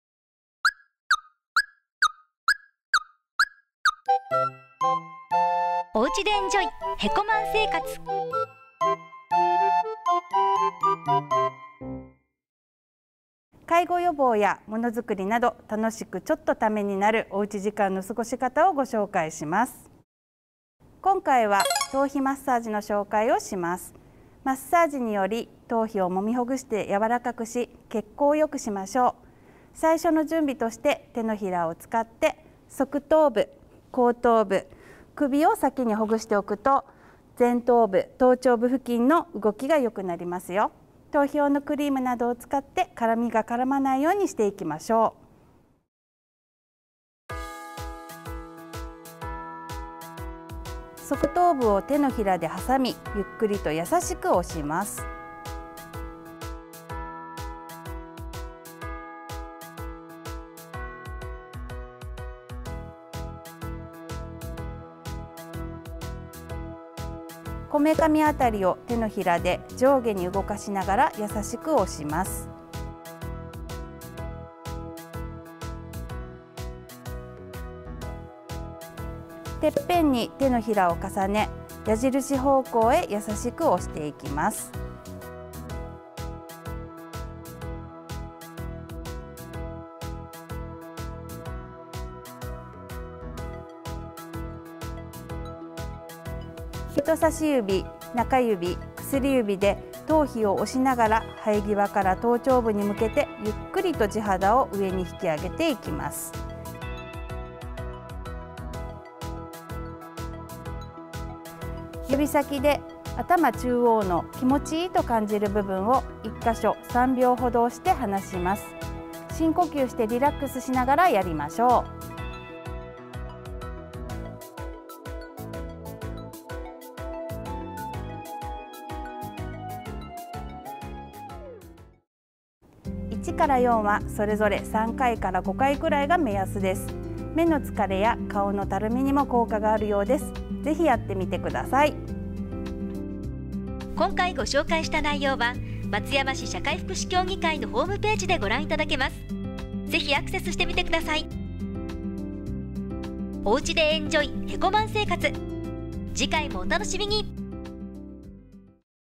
おうちでエンジョイ！へこまん生活 第3回「頭皮マッサージ」
介護予防や、ものづくりなど、楽しく、ちょっとためになる「おうち時間の過ごし方」をご紹介します。（制作協力：松山市社会福祉協議会）